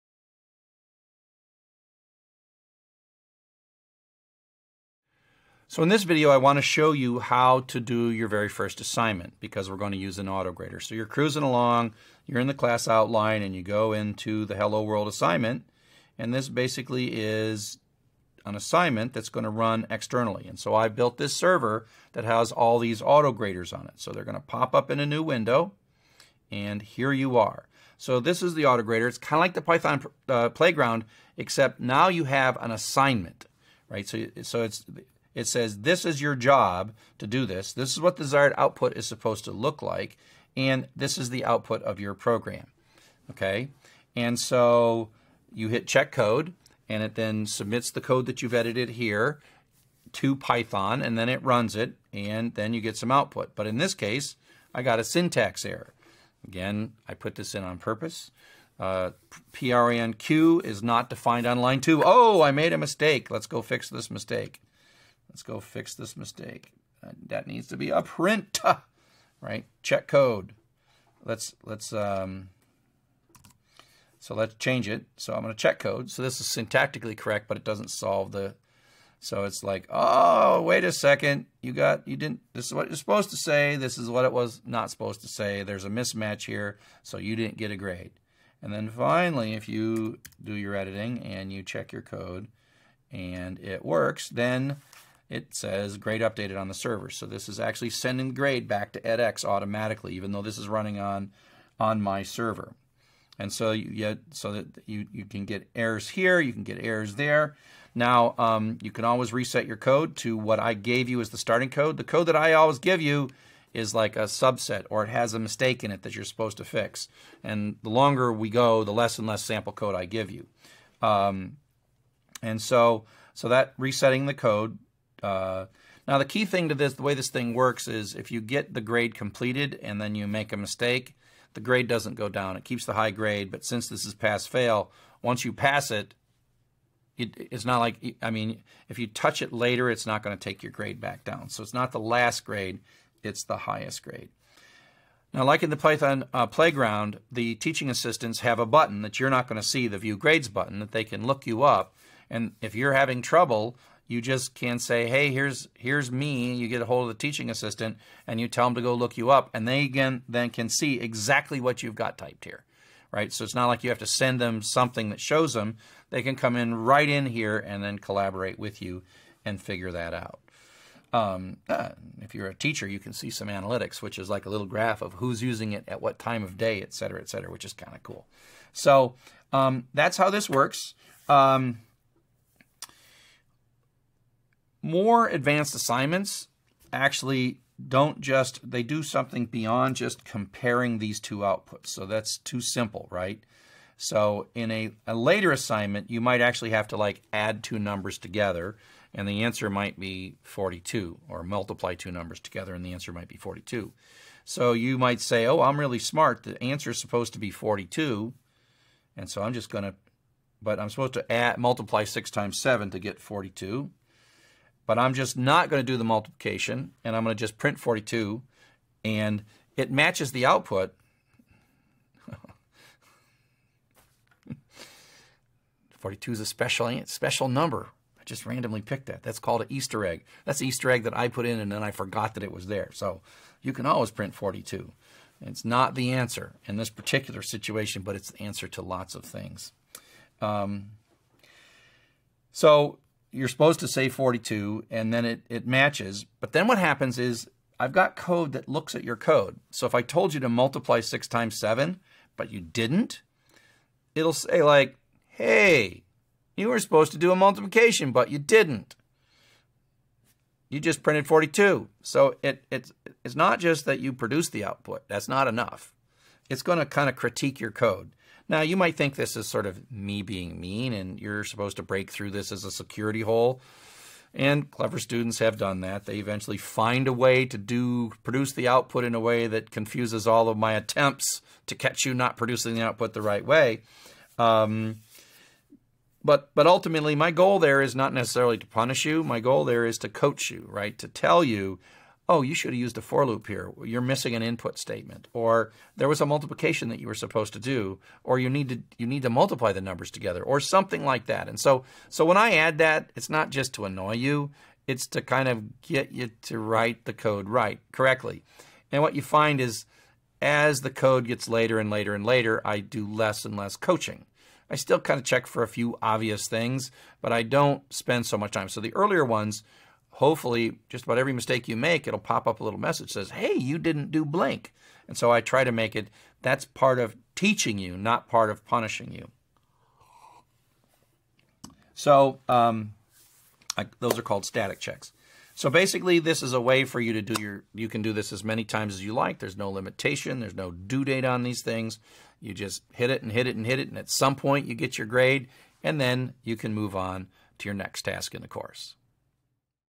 Video #3 Demonstration:Doing the HelloWorld. Programming for Everybody (Getting Started with Python)
Video # 3 Chapter One - Why We Program? Demonstration: Doing the "Hello World" Assignment. Programming for Everybody (Getting Started with Python).
You may find also this archived course "Programming for Everybody (Getting Started with Python)" on the (edX is an American massive open online course (MOOC) provider created by Harvard and MIT. See in detail about it on Wikipedia "edX".)

There is also the same Spanish course with Spanish subtitles, "Programación para todos (empezando con Python)" on the edX.